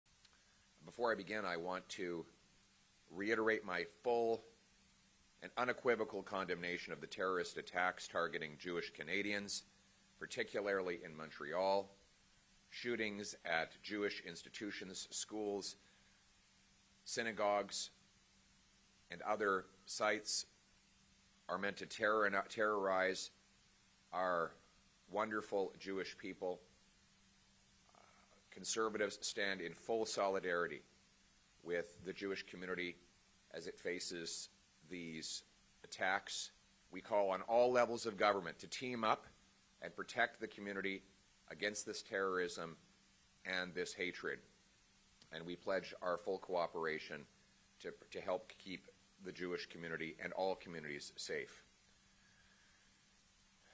Protect our Jewish community
The federal government must step up to protect our Jewish community against terrorists and hatemongers.